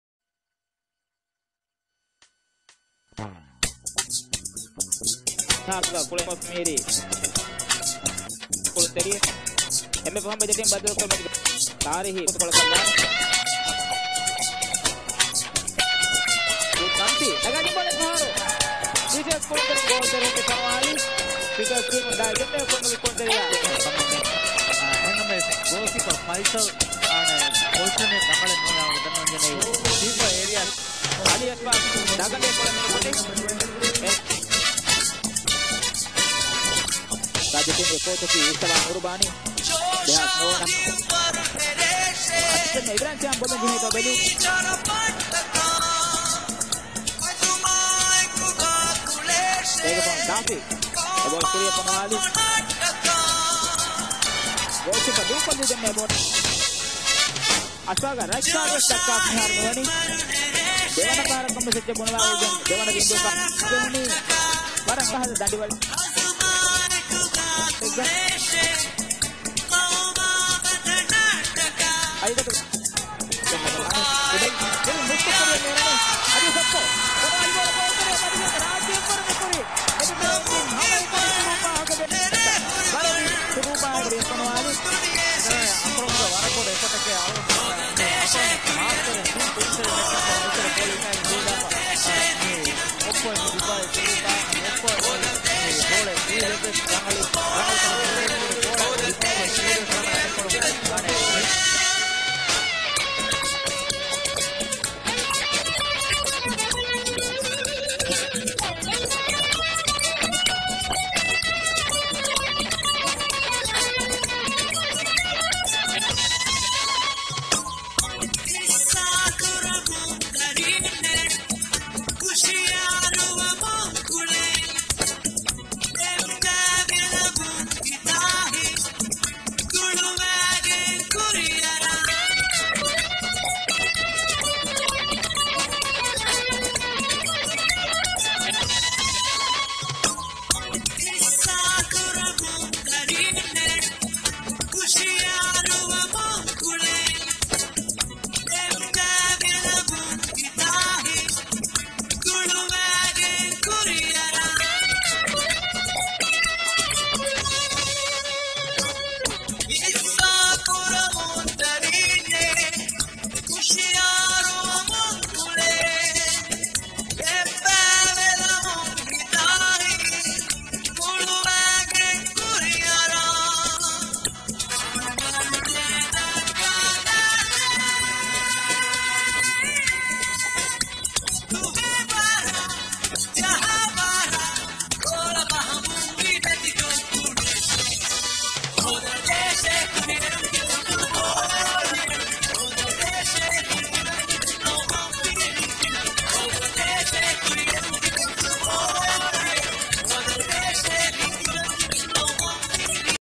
MALDIVES VS SEYCHELLES.mpg
MALDIVES VS SEYCHELLES MATCH 24-NOV-2011, MALE' MALDIVES
MALDIVES 2, SEYCHELLES 1